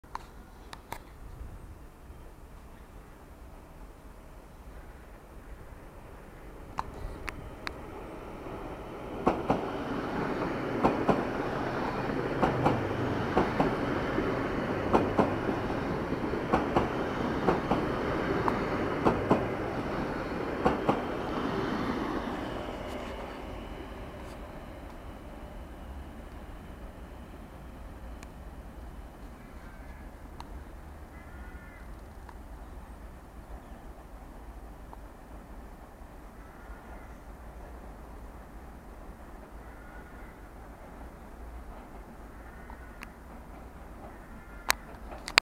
Łódzkie Trio
Przejazd trio Łódzkich Flirtów z pociągiem 9115 z Łodzi Kaliskiej do W-wy Wsch.